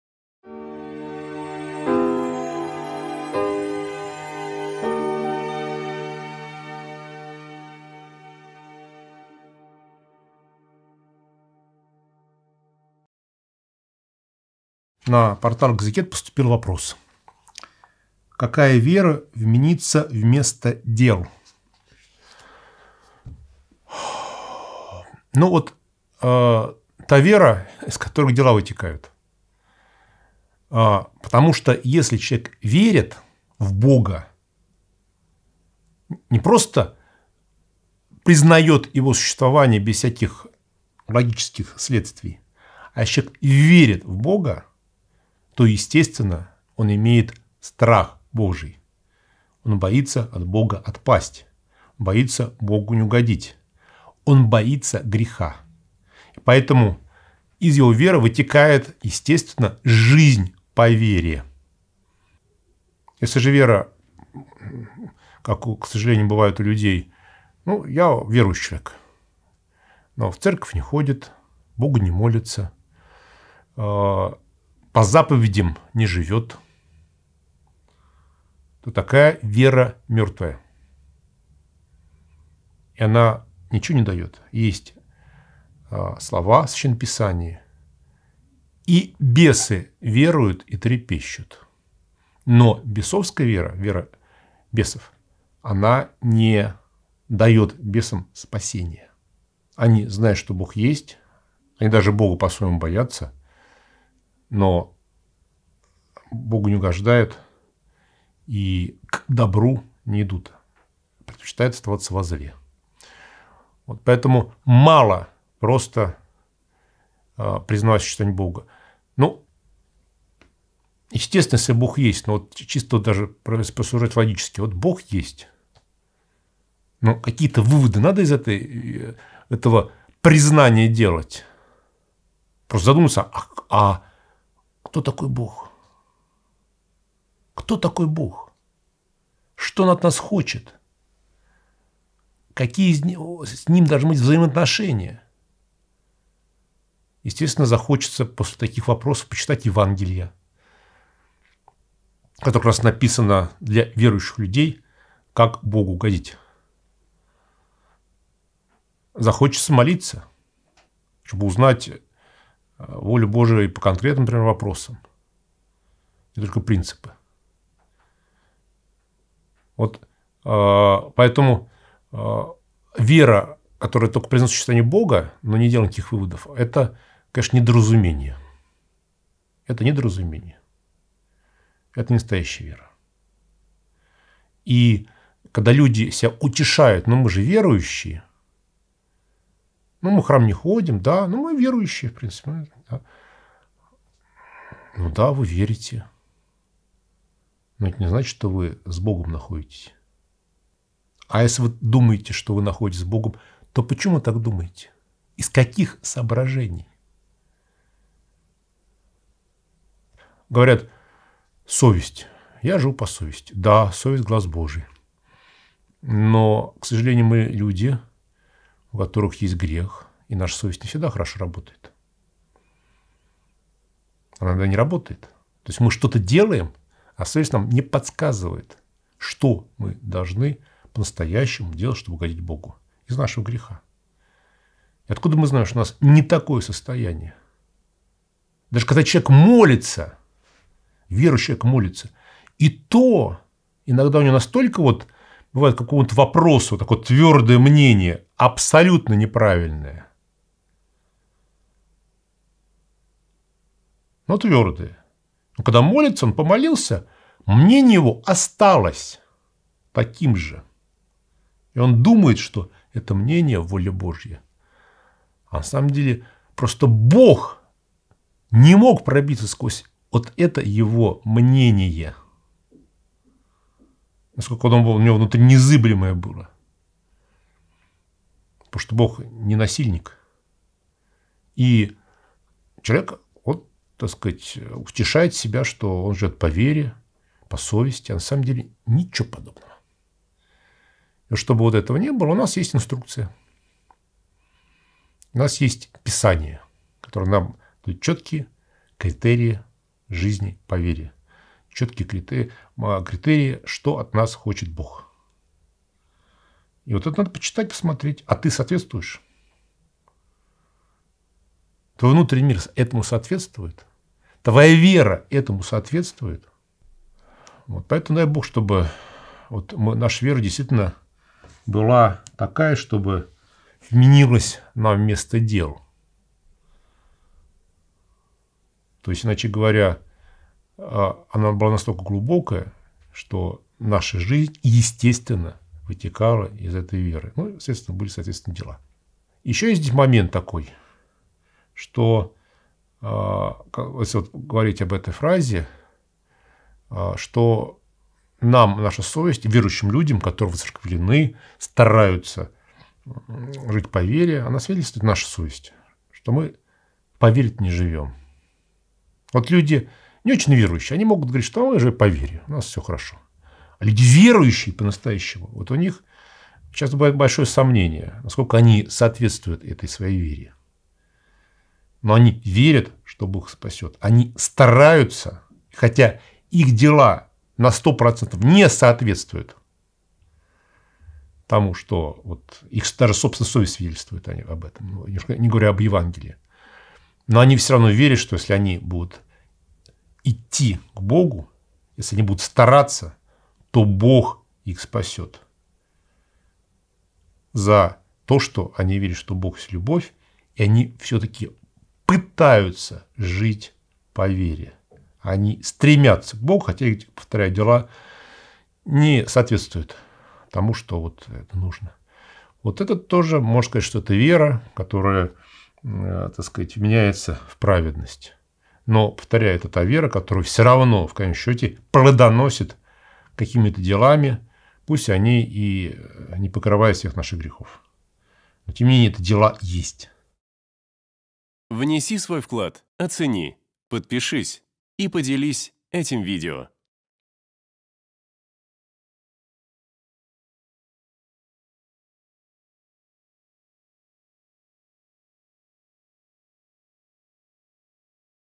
ВЕРА НАСТОЯЩАЯ И ВЕРА БЕСОВСКАЯ. Протоиерей Александр Никольский
Команда Экзегет.

В сегодняшнем видео на библейском портале Экзегет вместе с протоиереем Александром Никольским размышляем на тему: "Вера настоящая и вера бесовская."

🔸  Страх Божий
🔸  Жизнь по вере
🔸  Познание Бога
🔸  Молитва
🔸  Спасение